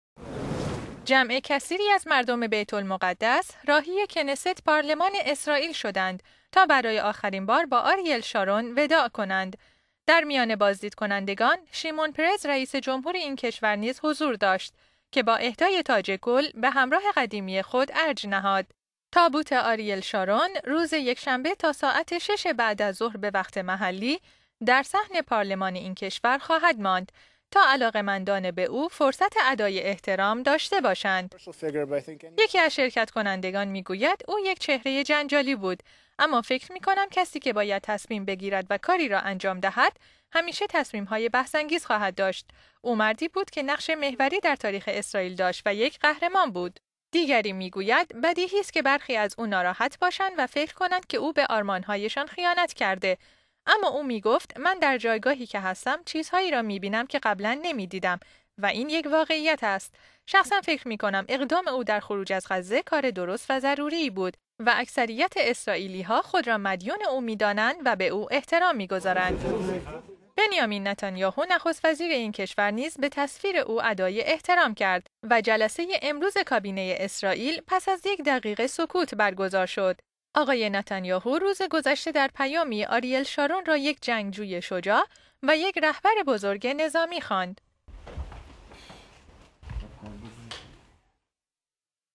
وداع مردم و دولتمردان اسرائیل با آریل شارون
جمع کثیری از  مردم بیت المقدس از صبح امروز راهی کنست،...

جمع کثیری از  مردم بیت المقدس از صبح امروز راهی کنست، پارلمان اسرائیل شدند تا برای آخرین بار با آریل شارون وداع کنند.

در میان بازدید کنندگان، شیمون پرز، رئیس جمهوری این کشور نیز حضور داشت که با اهدای تاج گل، به همراه قدیمی خود ارج نهاد.

تابوت آریل شارون، روز یکشنبه تا ساعت ۶ بعد از ظهر به وقت محلی در صحن پارلمان خواهد ماند تا علاقه مندان به او، فرصت ادای احترام داشته باشند.

یکی از شرکت کنندگان در این مراسم می گوید: «او یک چهره جنجالی بود اما فکر می کنم  کسی که باید تصمیم بگیرد و کاری را انجام دهد، همیشه تصمیم های بحث انگیز خواهد  داشت و او مردی بود که نقش محوری در تاریخ اسرائیل داشت و یک قهرمان بود.»

دیگری می گوید: «بدیهی است که برخی از او ناراحت باشند و فکر کنند که او به آرمانهایشان خیانت کرده است. اما او می گفت: «من در جایگاهی که هستم چیزهایی را می بینم  که قبلا نمی دیدم» و این یک واقعیت است. شخصا فکر می کنم اقدام او در خروج از غزه، کار درست و ضروری بود و فکر می کنم اکثریت بزرگی از اسرائیلی ها خود را مدیون او می دانند و به او احترام می گذارند.»

بنیامین نتانیاهو نخست وزیر اسرائیل نیز به تصویر او ادای احترام کرد و جلسه کابینه اسرائیل، امروز پس از یک دقیقه سکوت برگزار شد.